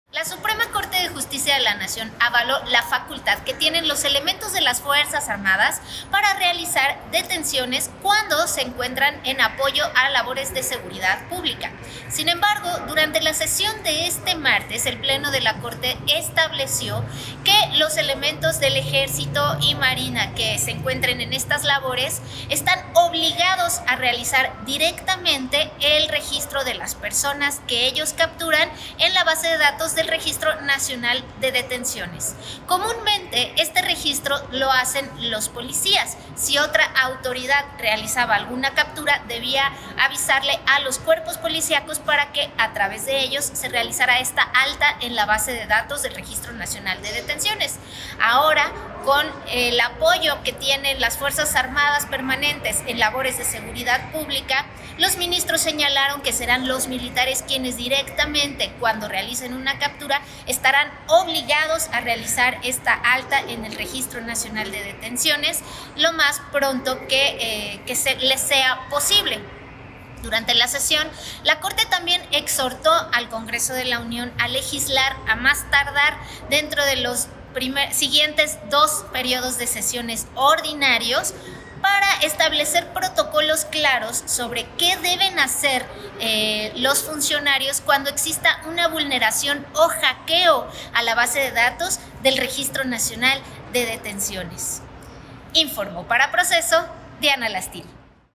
Las Fuerzas Armadas sí pueden hacer detenciones, resuelve la SCJN
Al resolver una controversia constitucional de la CNDH, la Suprema Corte avaló que las Fuerzas Armadas realicen detenciones de supuestos delincuentes.